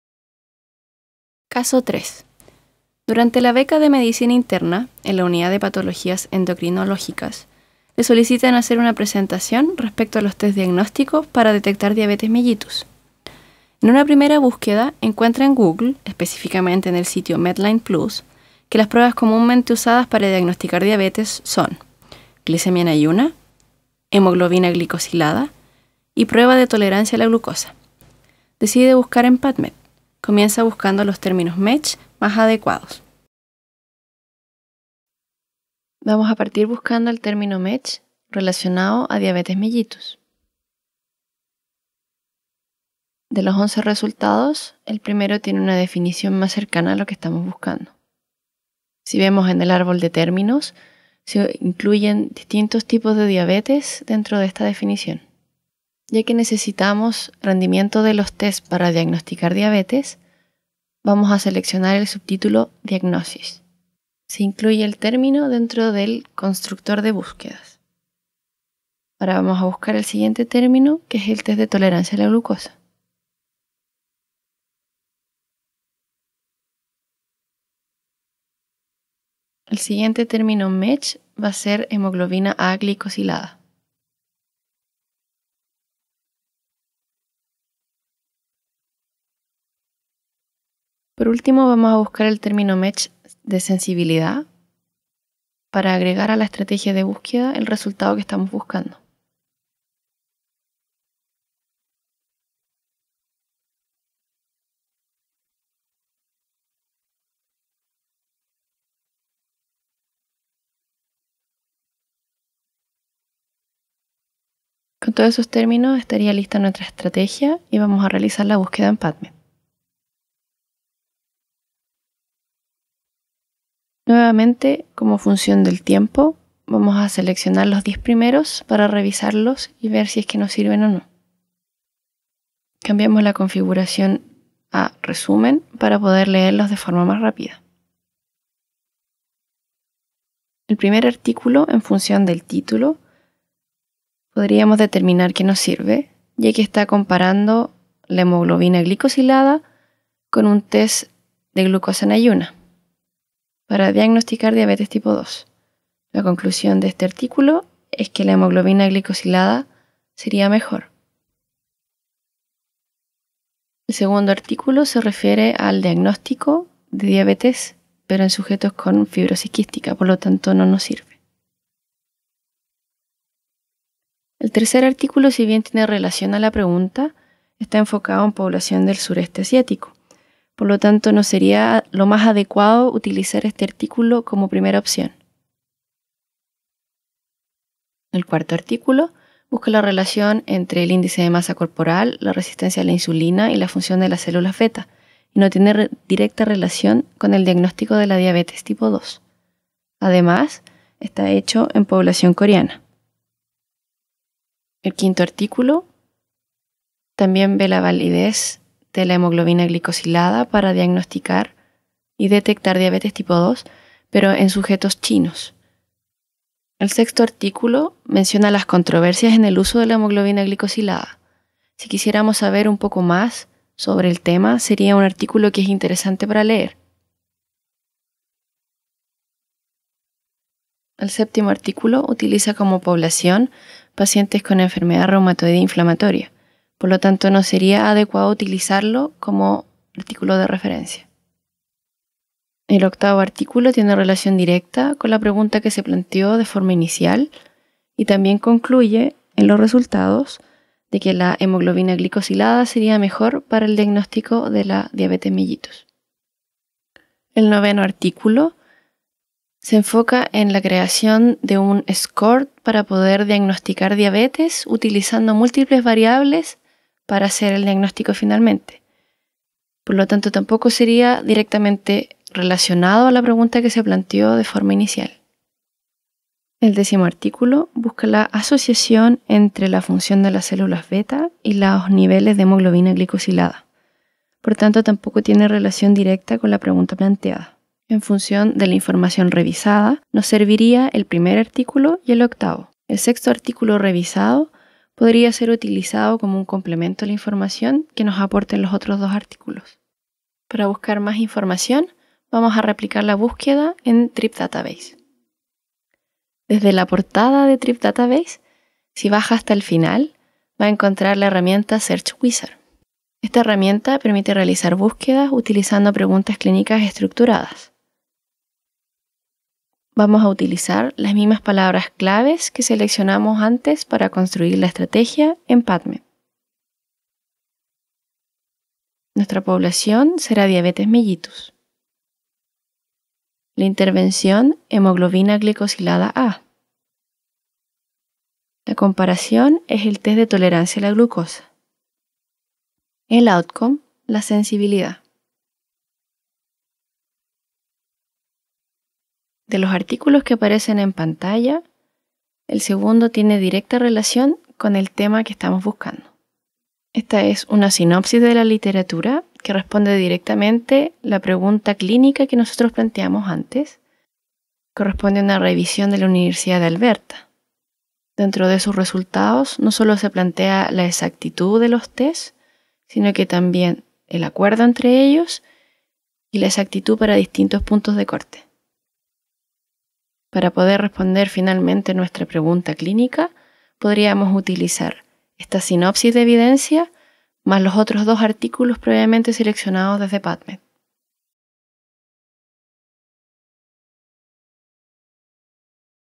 Tutorial UDD: Ejemplo 3- Criterios Diagnósticos de Diabetes (NA5)
Tutorial autodirigido para buscar literatura biomédica